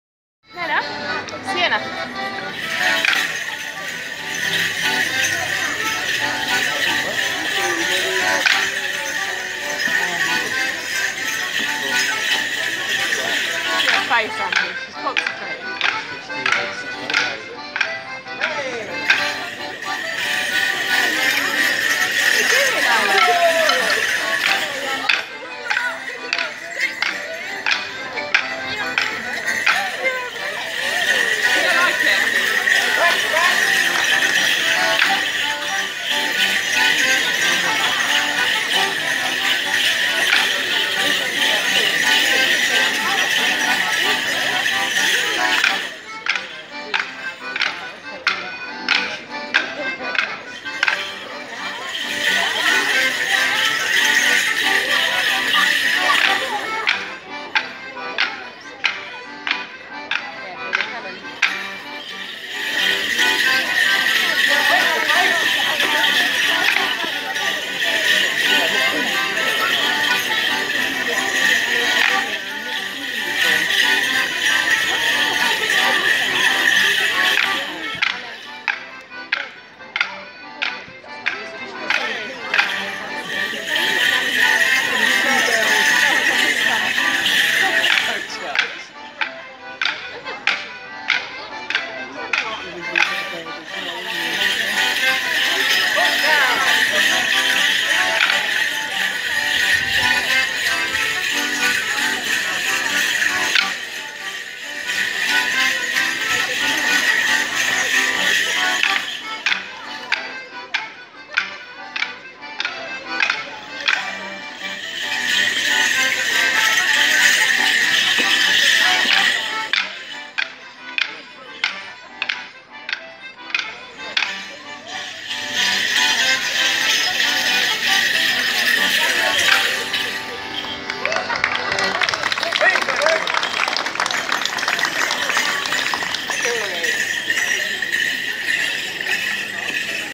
Bourne Borderers Morris, grupo tradicional de dança típica com bastões, região de Lincolnshire
Bourne Borderers Morris is a mixed side from Bourne in Lincolnshire, performing stick dances based on the few recorded traditional dances from the counties of Herefordshire, Shropshire, and Worcestershire along the Welsh Border, known as Welsh Border Morris